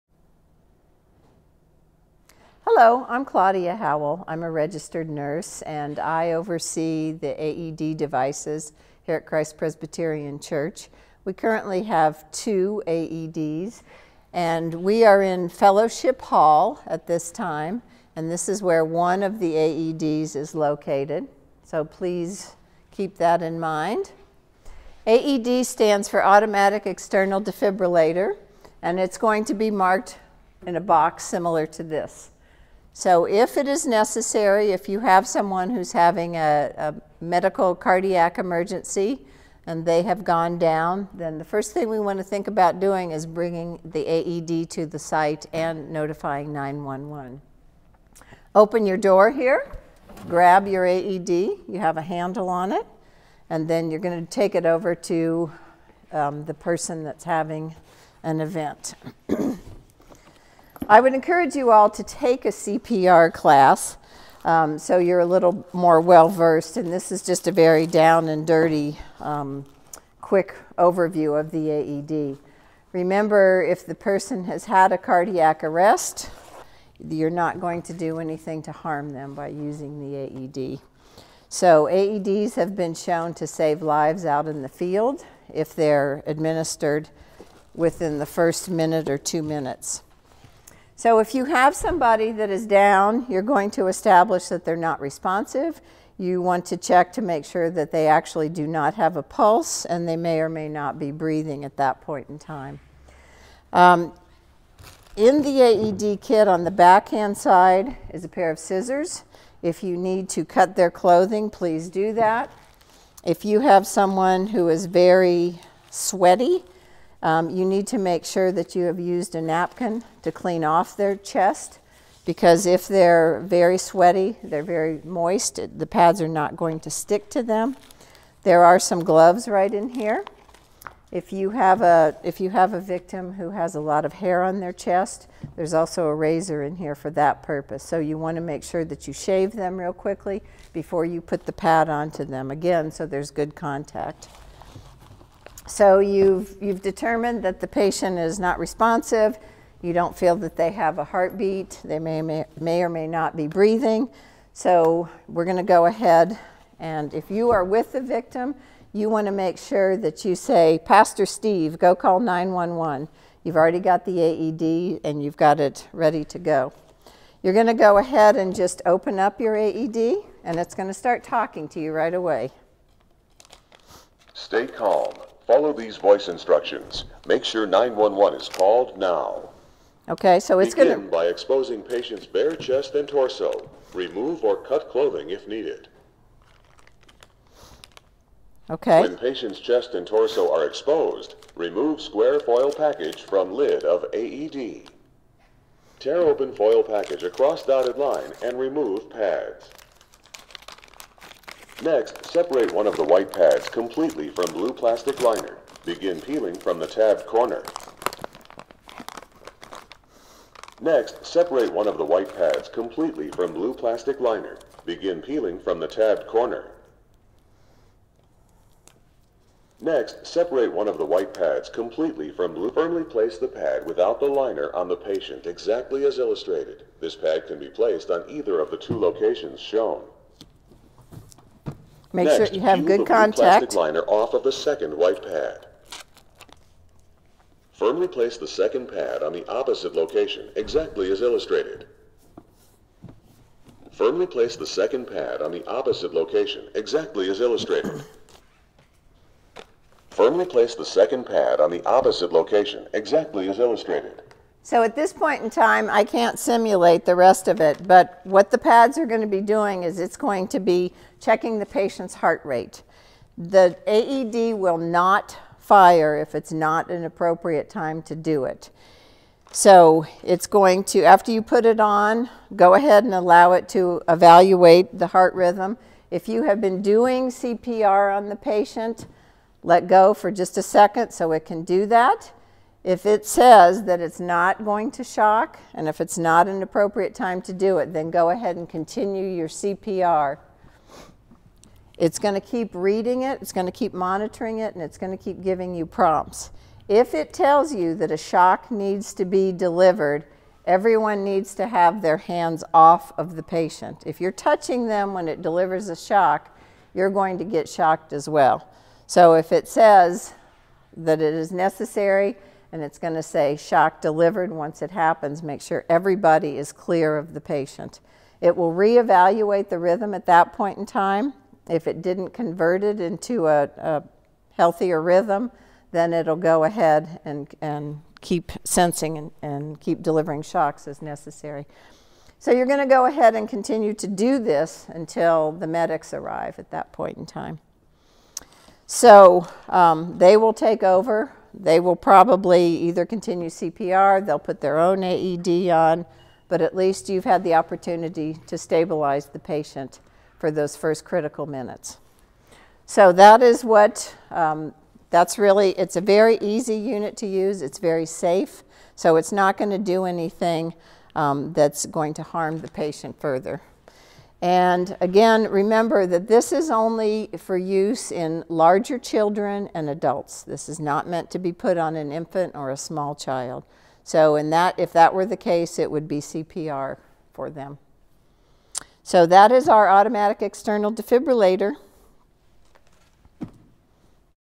AED device Training
A quick overview of how to use the AED device.  Be sure to find them on our campus so that you know where they are when they are needed.